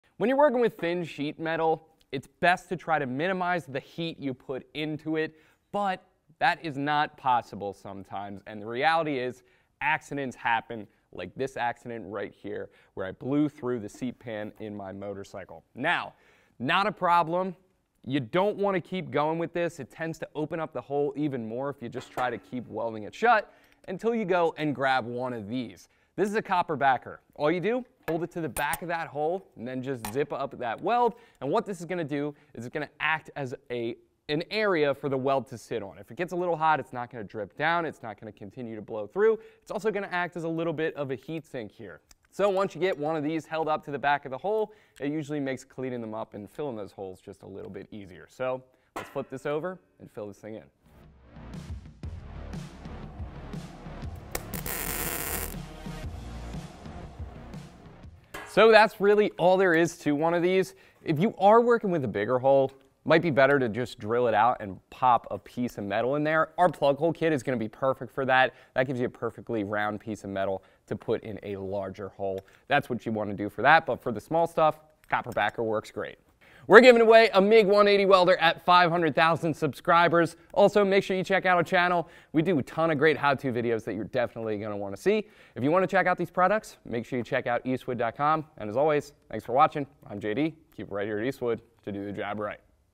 How to Fix Holes in Thin Metal When MIG Welding - Sheet Metal Tips and Tricks
In this Video JD goes over why you should always keep a pack of copper backers in your welding cart!

Copper "Welder's Helpers" plug holes as they support your welding in tight spots
Use these three 100%-copper "spoons" to back-up small holes, rips or voids between flat, round and angled panels while you weld.

90-Degree Angle Plate
Curved Plate
Offset Flat Plate